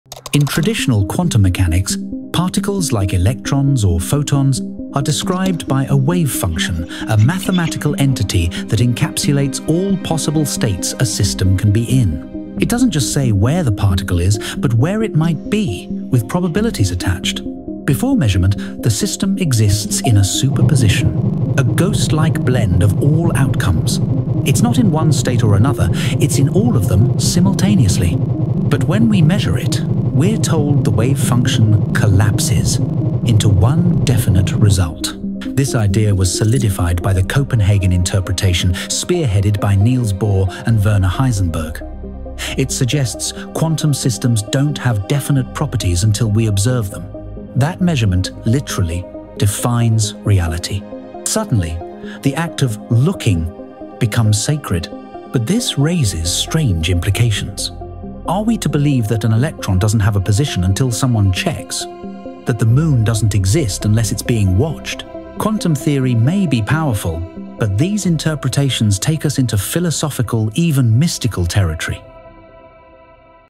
What Is Wavefunction Collapse?
In quantum mechanics, we’re told particles exist in all states until measured.
But does observation really define reality?
This short explores the eerie idea of wavefunction collapse — and the philosophical rabbit hole it opens.